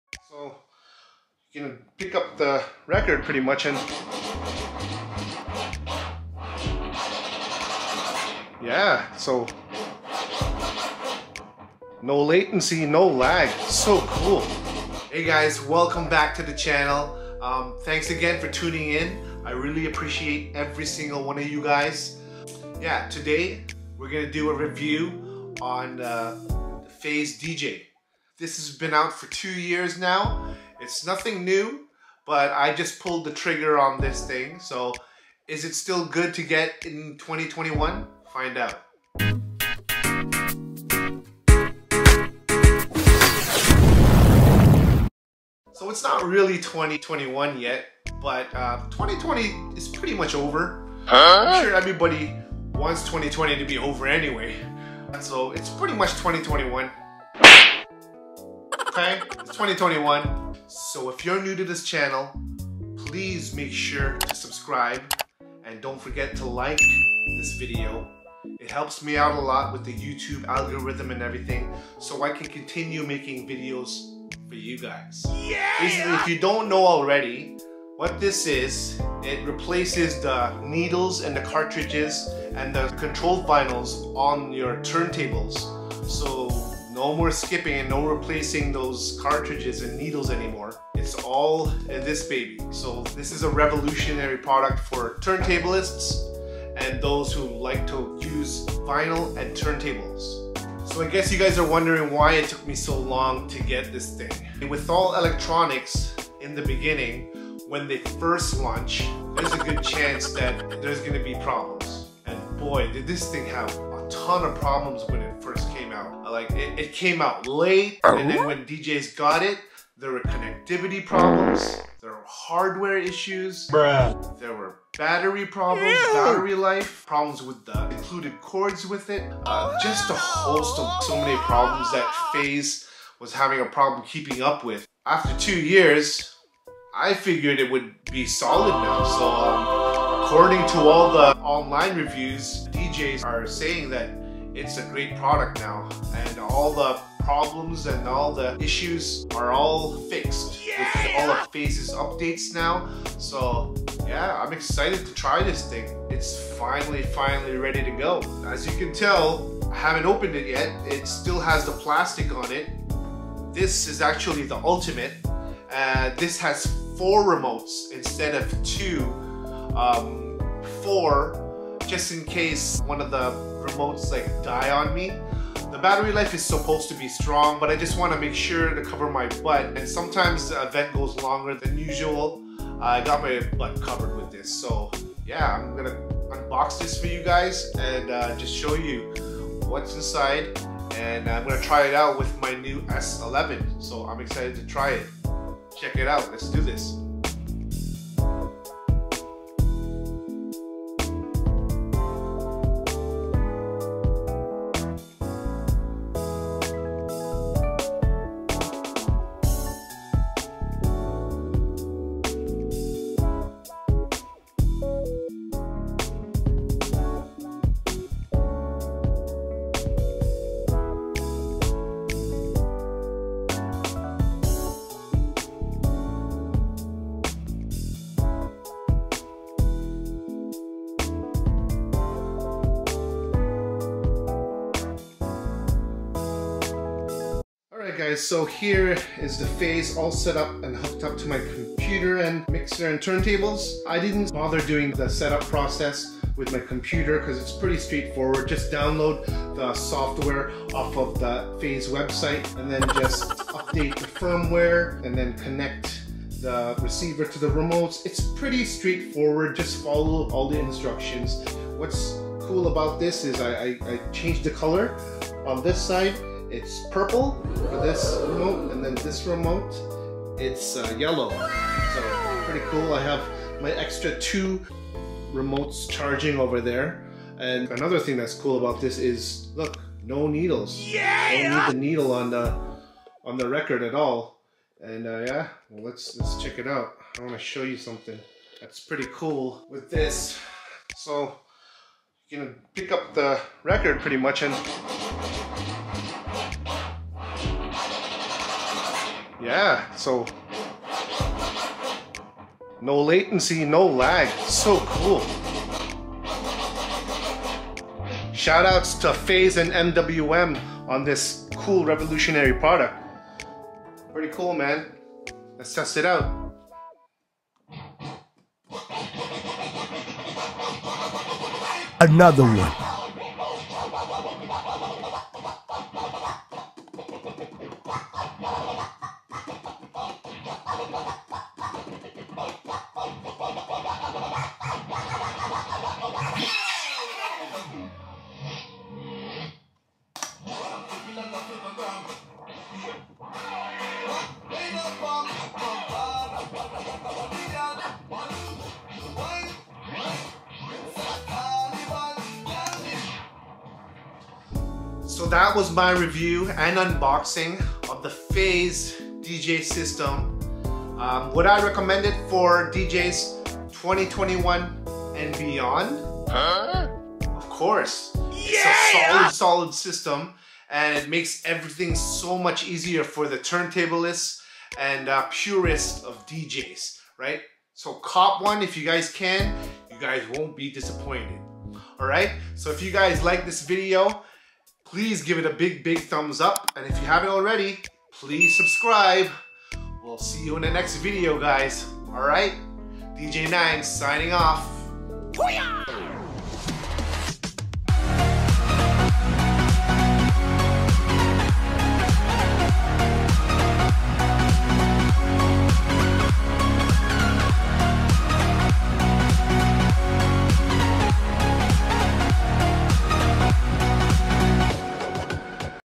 IS IT STILL WORTH BUYING IN 2021?/PHASE DJ/DJ GEAR REVIEW
Hey guys this is a video review/unboxing on the phase dj system. I explain why in more detail in the video. This is my honest review of this system. Appreciate you!